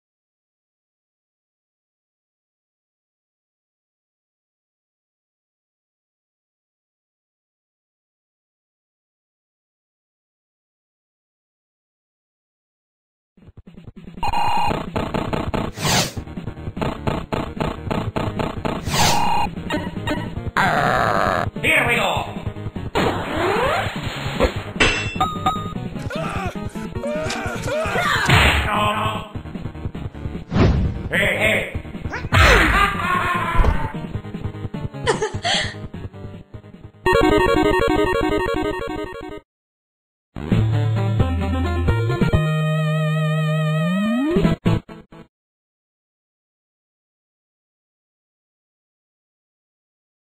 Flash Animation: Batwoman - The Animated Shorts - Episode 6
The 6th episode of my Batwoman shorts

Plot: After The Penguin got away from the cops after he robbed a bank, he doesn’t expect Batwoman to screw him over since he thought he could escape easily. But that immediately changed and a small standoff between the bat and bird come to blows.

The new costume is based off “The Long Halloween” story arc and the plot is influnced from BATMAN Unlimited

Enjoy, my friends... Animation made with Adobe Flash CS6 and CC